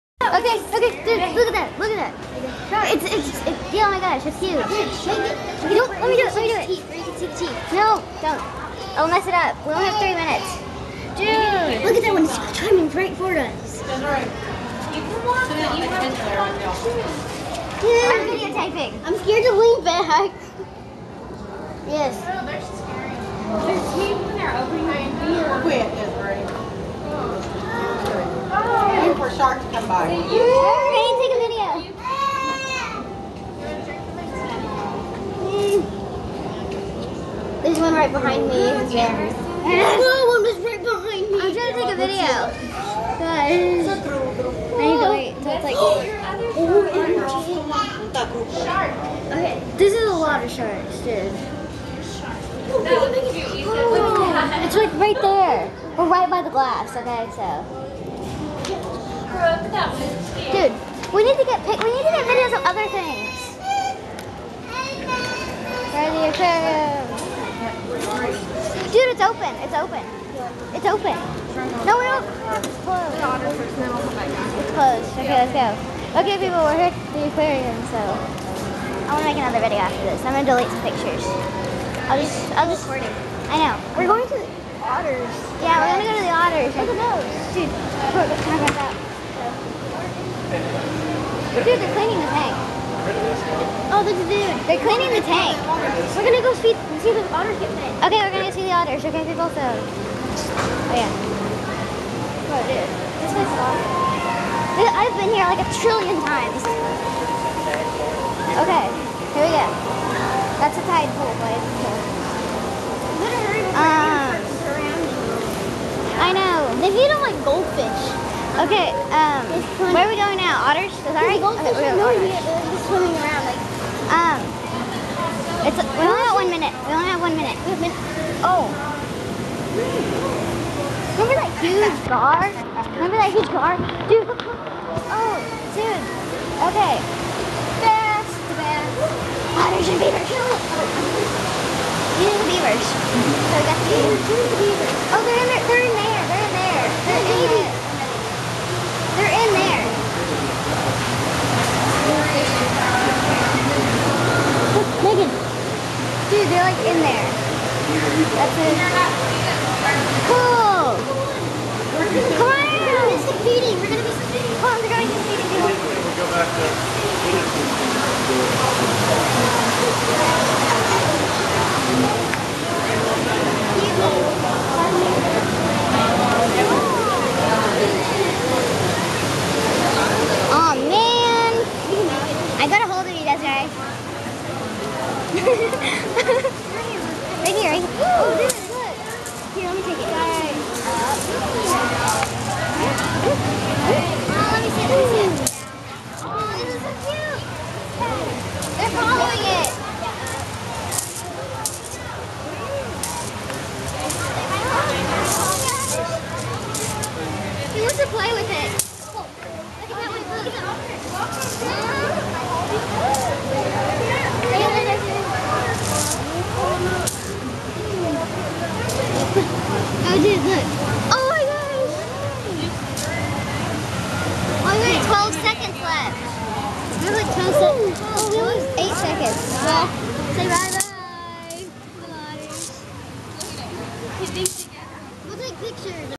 Trip to the Aquarium Part 1
going to the aquarium with Desiree